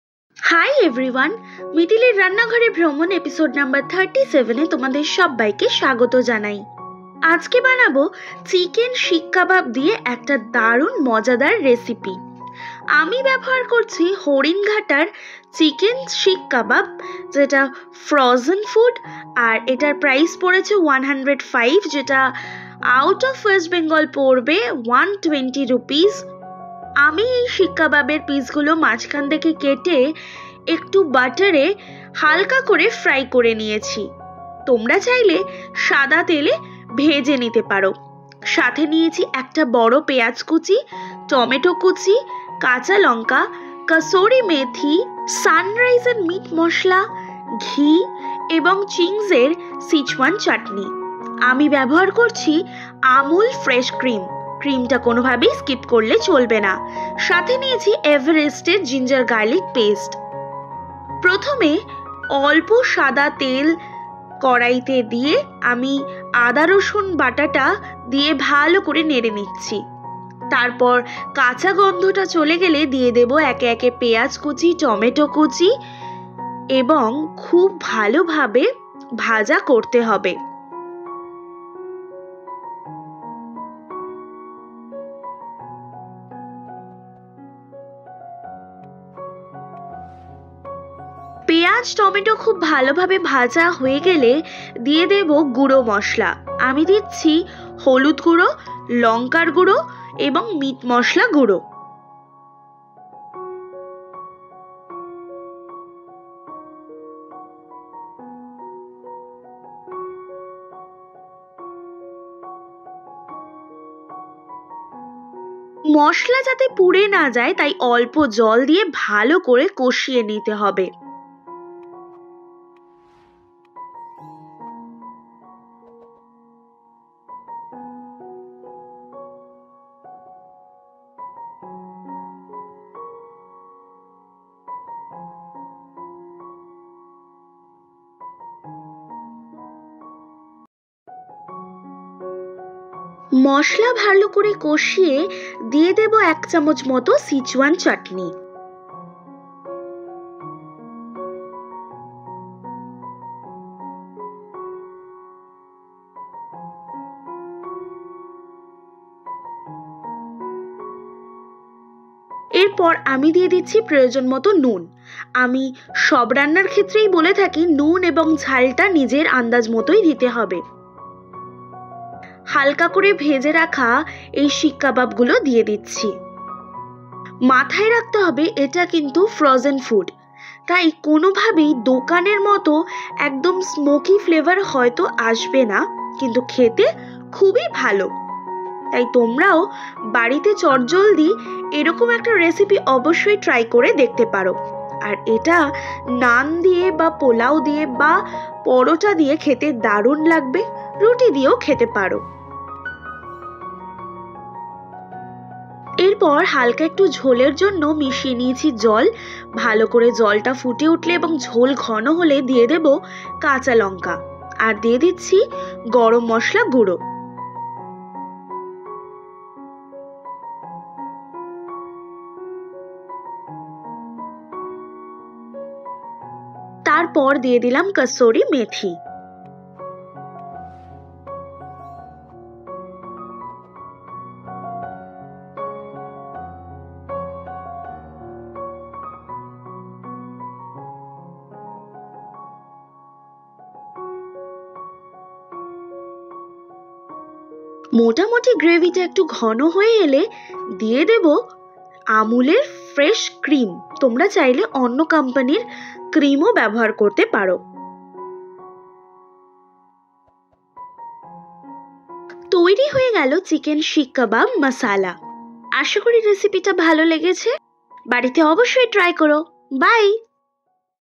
VLOG 125 চটপট চিকেন শিক কাবাব মশলা🤤
উপকরণ (Ingredients)⬇️

*চিকেন শিক কাবাব-এক প্যাকেট  Haringhata Chicken Seekh Kebab (1 packet)
*আদা রসুন বাটা Everest Ginger Garlic paste( 1 packet)
*পেঁয়াজ Onion
*টমেটো Tomato
*কাঁচালঙ্কা Green Chilli
*মাখন (কাবাব হাল্কা ফ্রাই করার জন্য) Butter (as required)
*সাদা তেল Refined oil
*হলুদগুঁড়ো Turmeric Powder
*শুকনো লঙ্কা গুঁড়ো Red Chilli Powder
*মীট মশলা গুঁড়ো Meat Masala Powder
*গরম মশলা গুঁড়ো Whole spices masala
*কসৌরি মেথি Kasuri Methi
*সিচুয়ান চাটনি Schezwan Chutney (1spoon)
*আমুল ফ্রেশ ক্রিম Amul fresh cream (as required)
*ঘী  Ghee (half teaspoon)
*নুন Salt (as required)

[বি.দ্র: রান্নায় ব্যবহৃত ঘী এর কথা আমি ভিডিওতে বলতে ভুলে গেছি।হাফ চামচ ঘী ব্যবহার করেছি,চাইলে বাদ দেওয়া যায়]